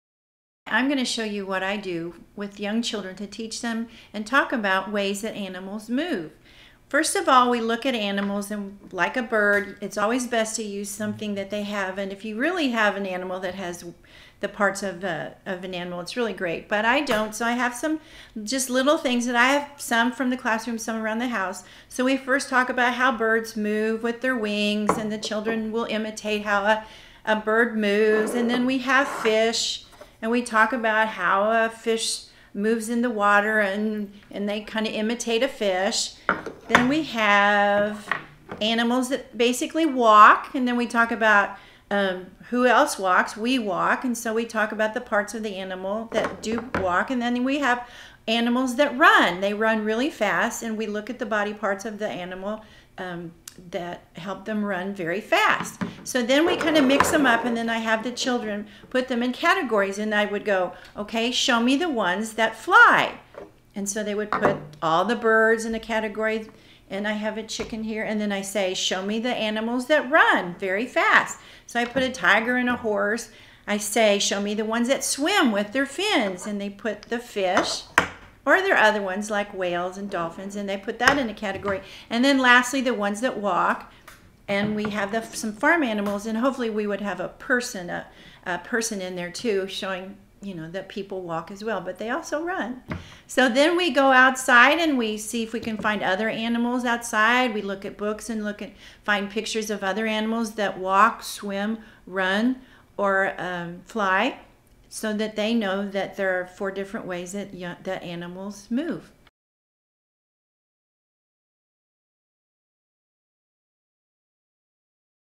How Animals Move
Neva Thiessen  |  Early Childhood Educator  |  Wichita, KS

Using figures, toys, or pictures, the teacher and students discuss the ways different animals move: swimming, flying, walking, and running. Then, students practice by sorting each animal into groups by type of movement.